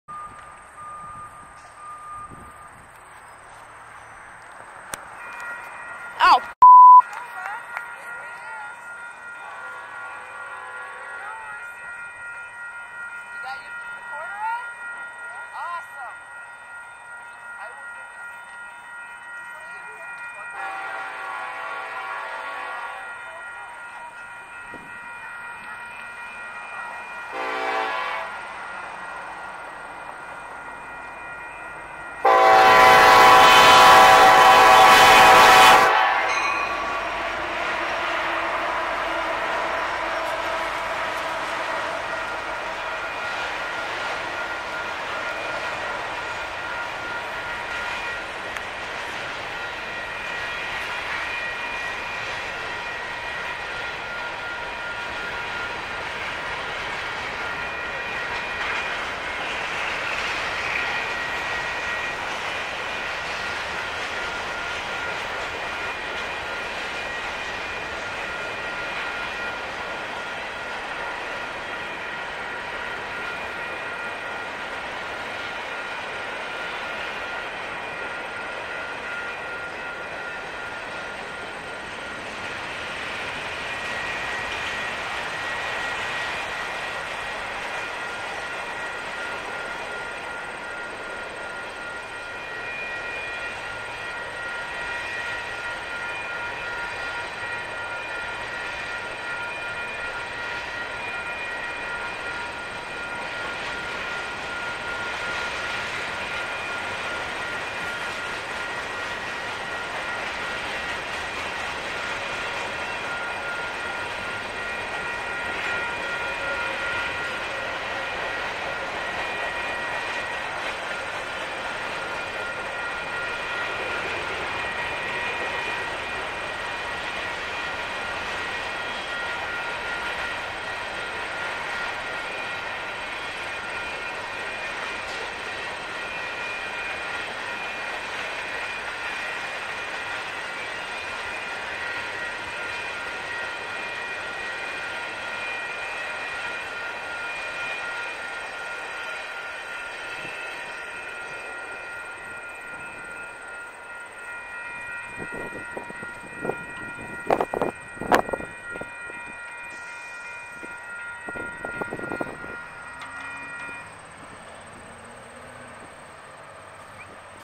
MPRVP-25 through Sono Junction 7/27/18
Wasn't expecting this. We couldnt hear the train so we were about to leave. Another train came through about 15 minutes earlier and we missed that. All of a sudden the crossings came to life. Scared the crap out of me but okay!